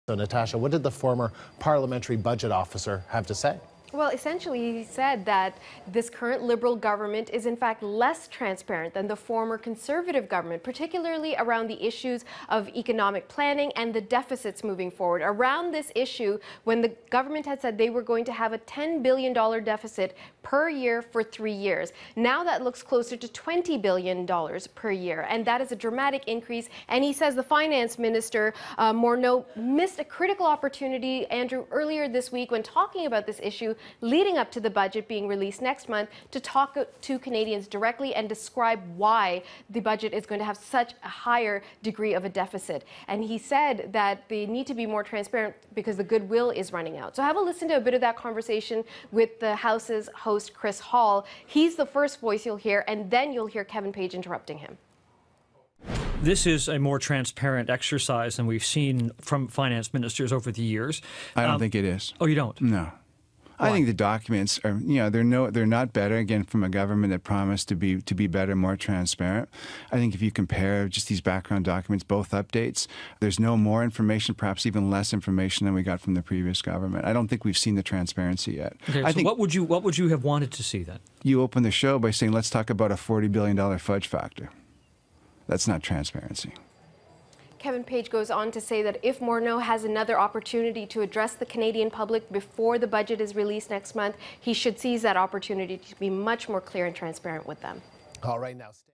Kevin Page says Trudeau Liberals no more transparent than Harper
Former Parliamentary Budget Officer Kevin Page tells CBC on February 27, 2016, that the Justin Trudeau government is no more transparent than the Harper Conservative government when it comes to budgets and fiscal planning.